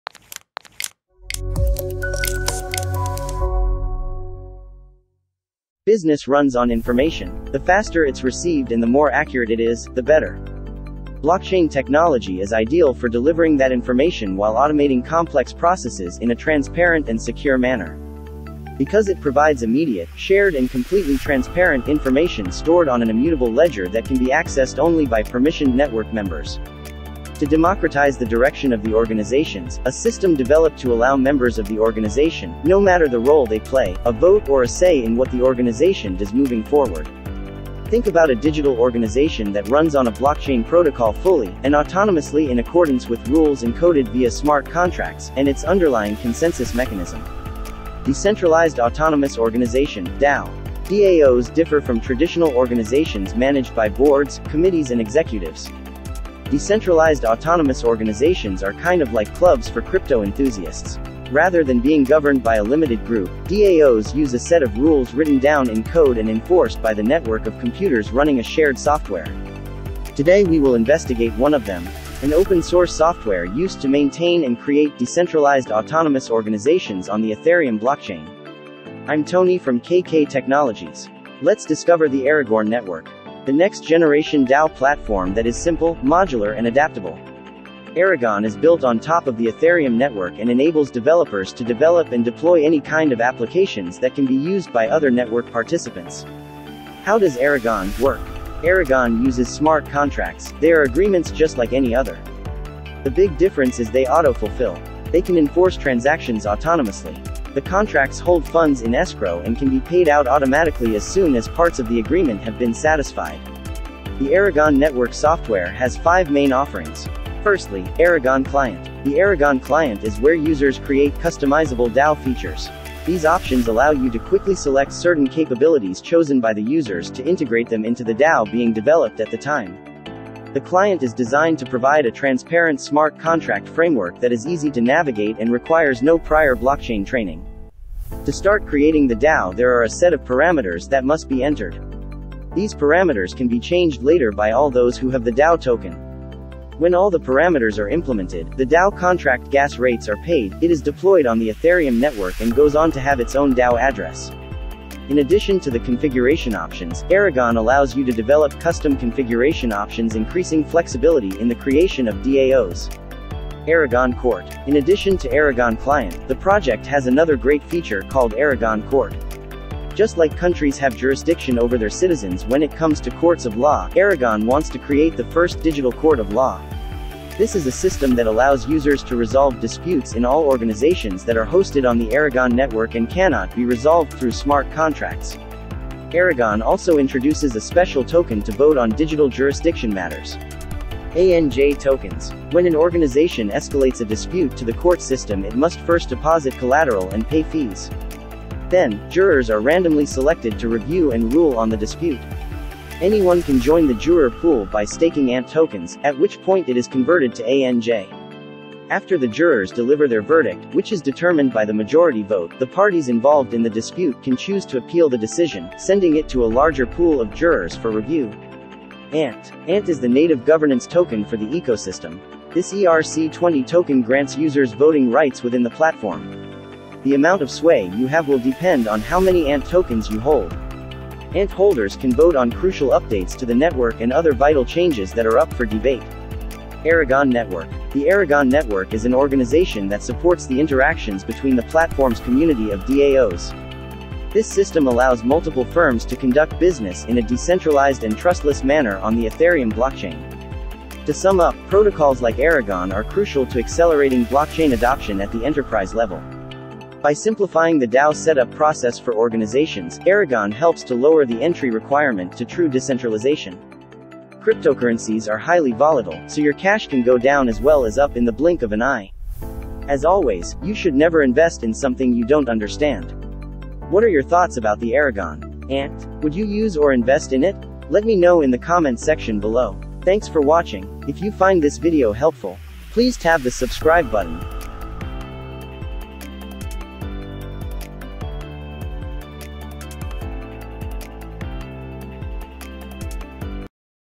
What is ARAGON - ANT Explained
Business runs on information. The faster it’s received and the more accurate it is, the better. Blockchain technology is ideal for delivering that information while automating complex processes in a transparent and secure manner. Because it provides immediate, shared and completely transparent information stored on an immutable ledger that can be accessed only by permissioned network members.

    To  democratize the direction of the organizations, A system developed to allow members of the organization—no matter the role they play—a vote or a say in what the organization does moving forward.
    Think about a digital organization that runs on a blockchain protocol fully, and autonomously in accordance with rules encoded via smart contracts, and its underlying consensus mechanism.

“Decentralized autonomous organization”, DAO.

DAO’s differ from traditional organizations managed by boards, committees and executives. Decentralized autonomous organizations are kind of like clubs for crypto enthusiasts. Rather than being governed by a limited group, DAOs use a set of rules written down in code and enforced by the network of computers running a shared software.

In this video  we will investigate one of them. An open-source software used to maintain and create decentralized autonomous organizations on the Ethereum blockchain.

 Let's discover the Aragon Network.